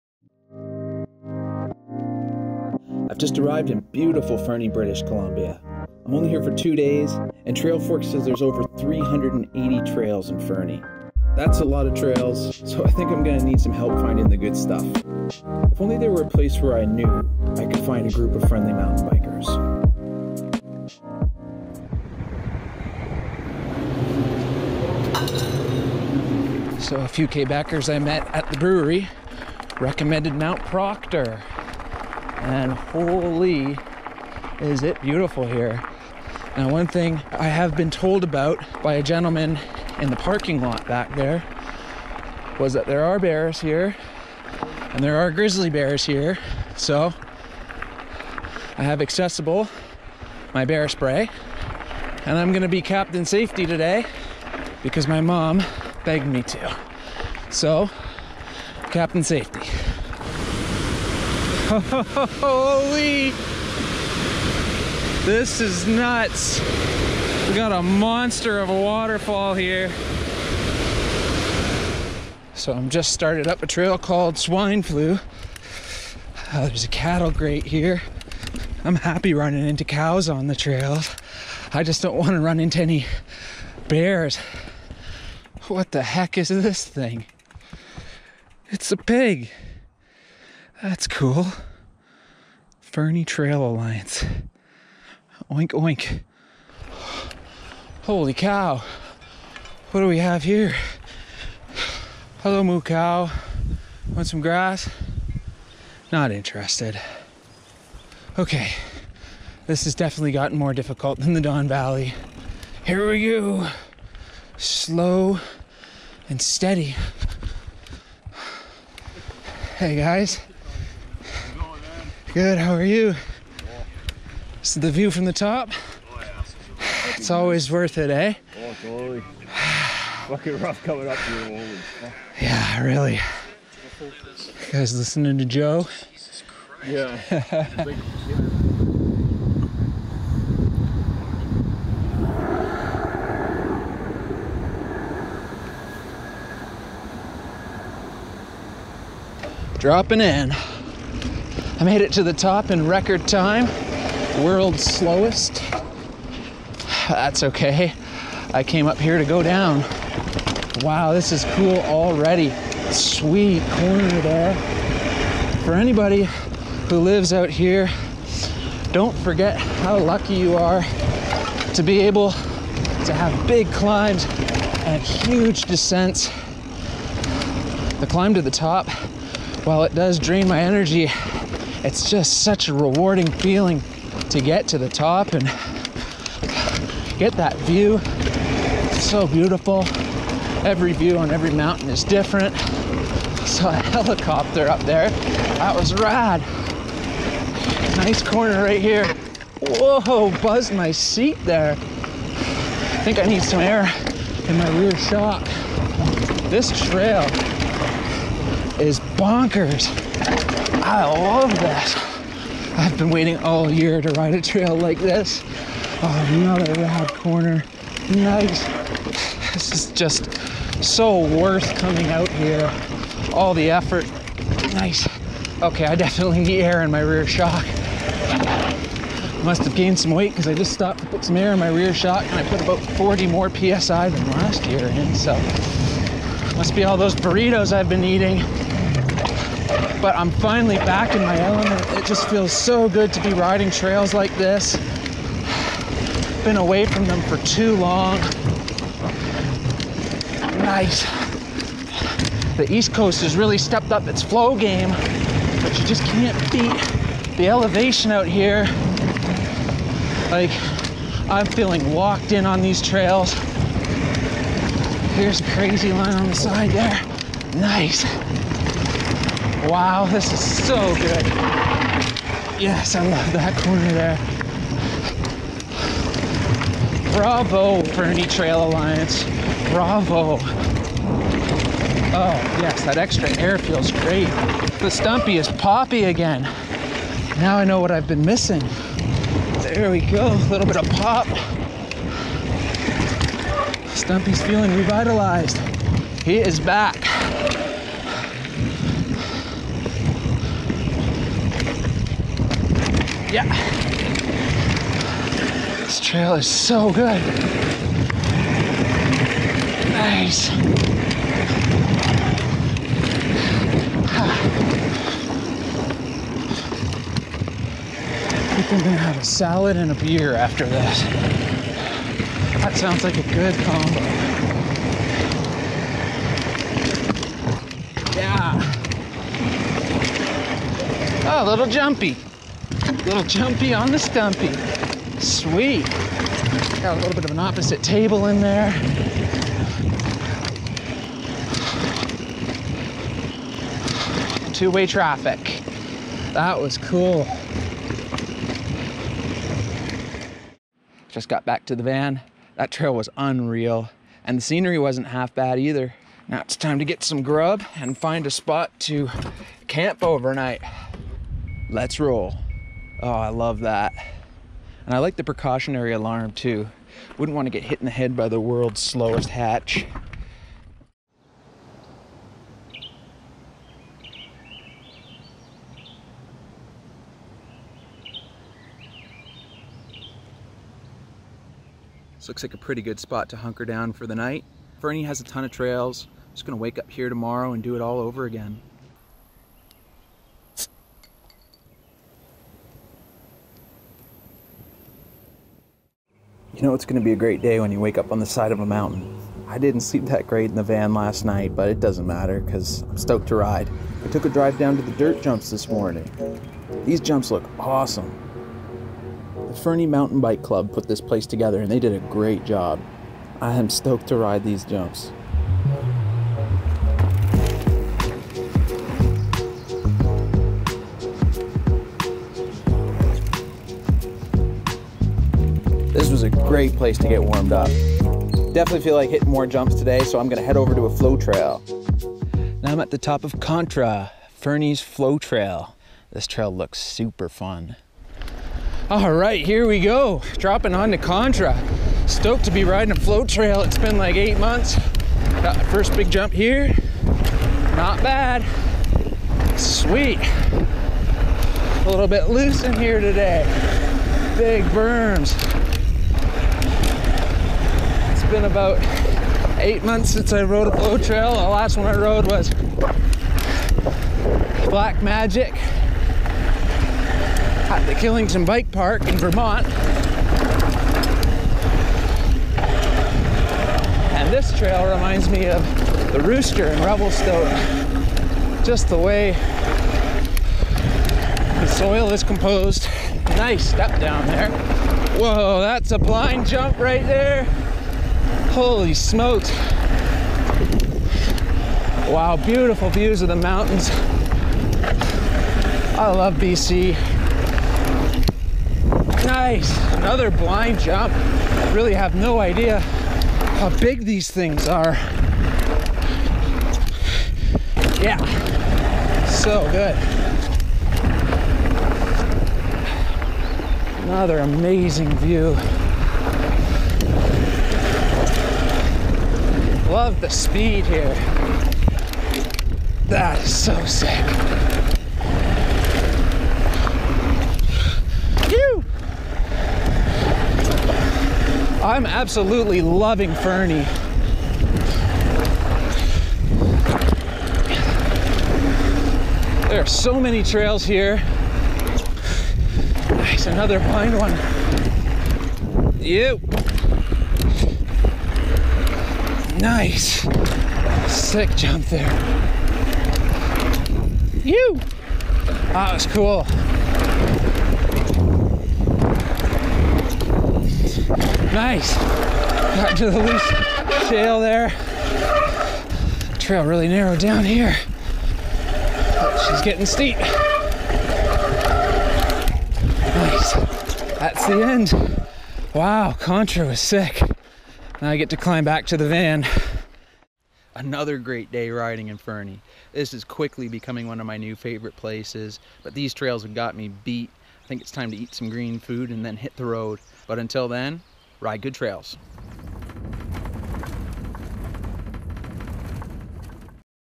Fernie is PACKED with trails! How do I choose? Specialized Stumpjumper
Fernie is a true mountain biking town.  Fernie is home to multiple sanctioned mountain bike trail networks as well as a bike park!  The bike park was not open while I was in town so pedaled my Specialized Stumpjumper up to top.  I spent 2 days in Fernie and had a great time!

Ride at your own risk! (but have fun)